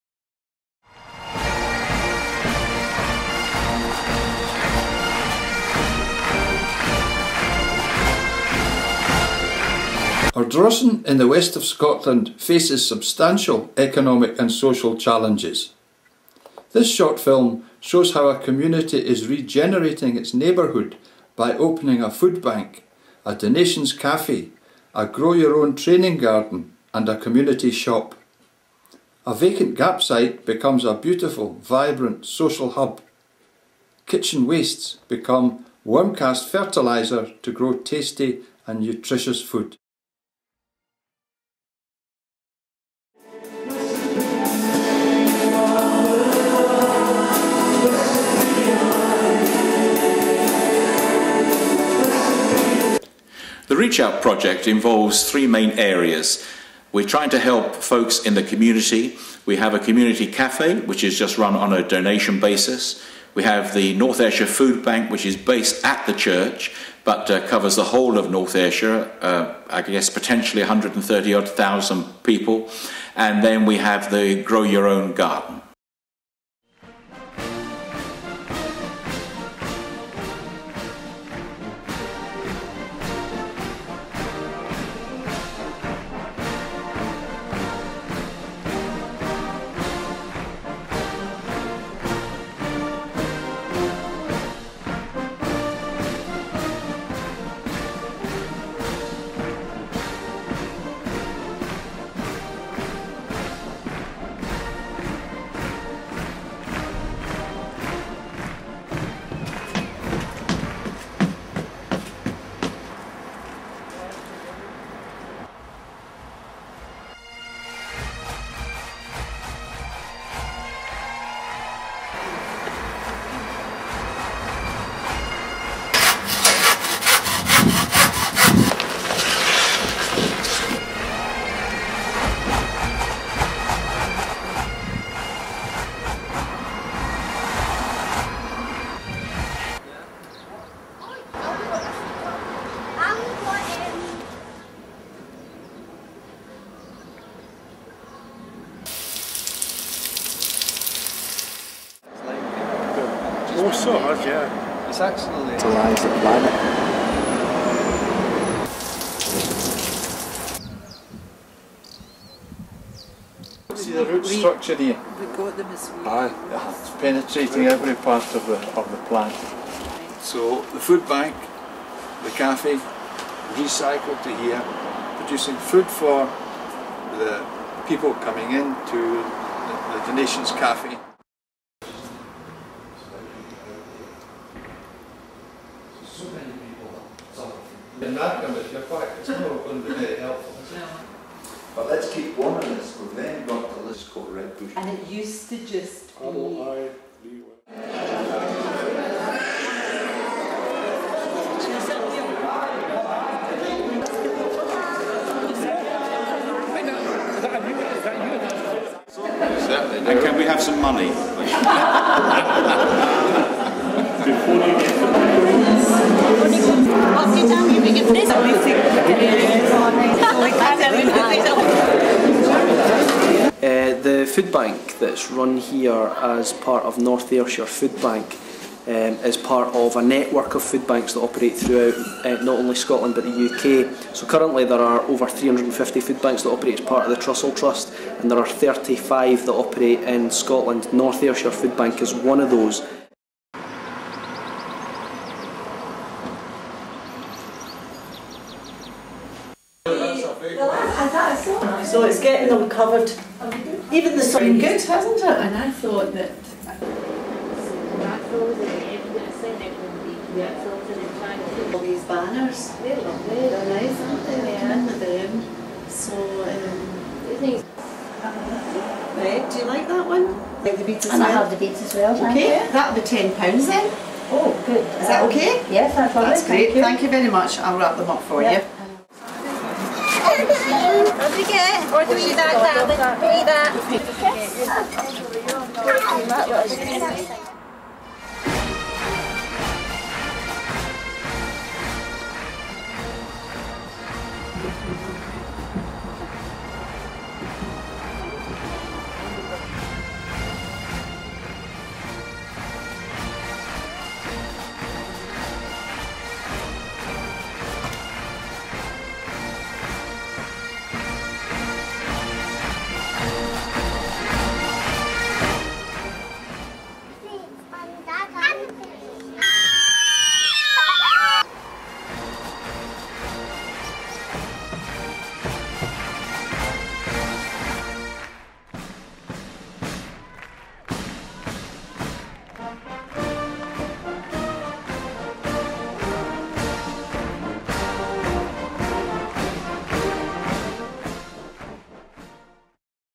Ardrossan Nazarene Garden Project
A film about economic and social regeneration in the small seaside town of Ardrossan, North Ayrshire, Scotland.
Featuring a donations café, food bank, organic garden and coffee /crafts shop.